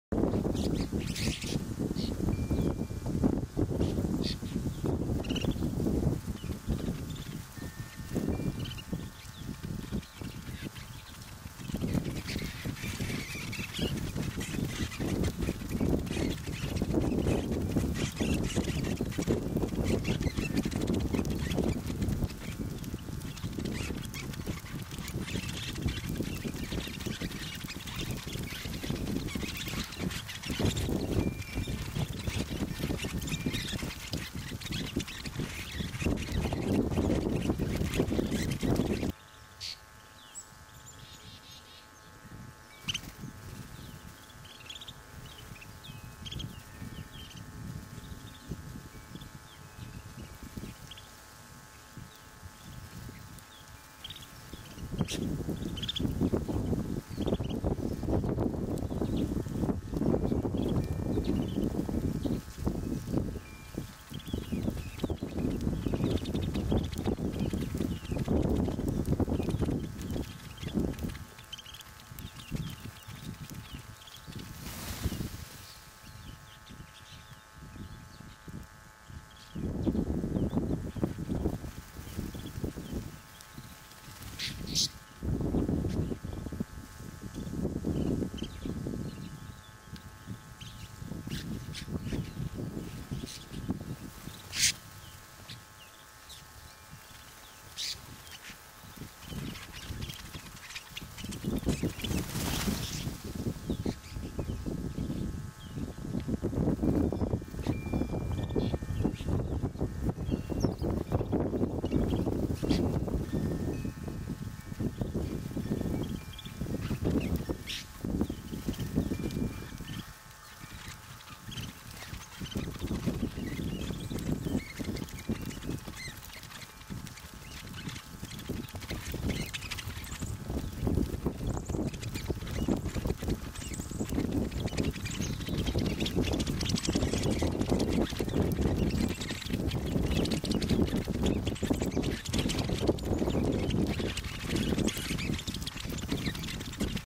Snow Buntings
Snow Buntings flying down from a tree.